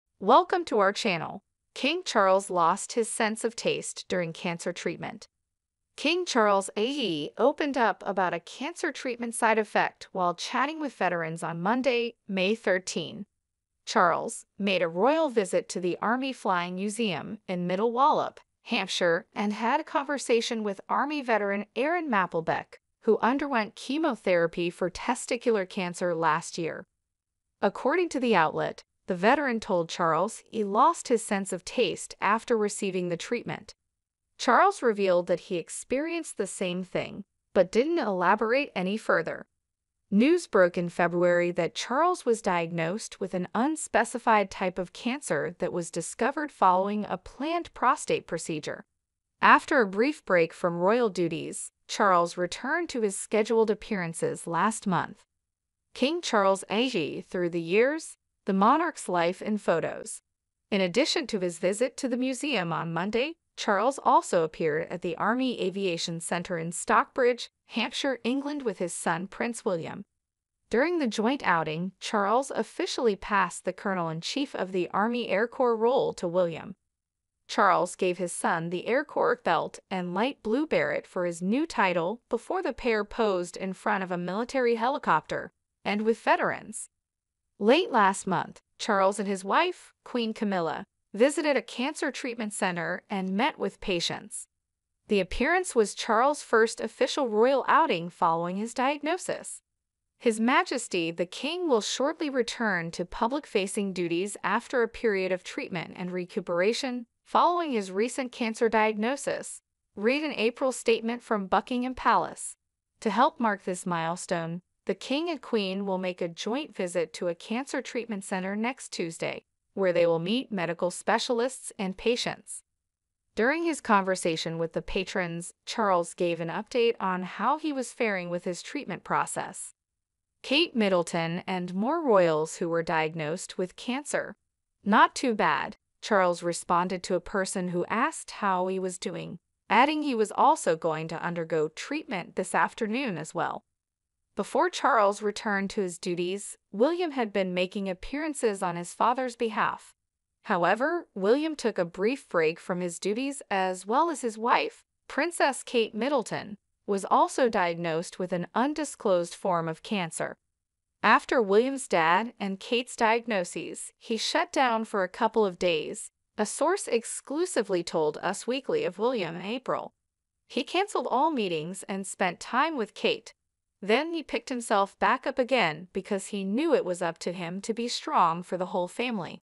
King Charles opened up about a cancer treatment side effect while chatting with veterans.
King Charles III revealed that he lost his sense of taste during cancer treatment. He visited the Army Flying Museum and had a conversation with army veteran Aaron Mapplebeck. Charles was diagnosed with an unspecified type of cancer in February. He returned to public-facing duties last month and visited a cancer treatment center with his son Prince William.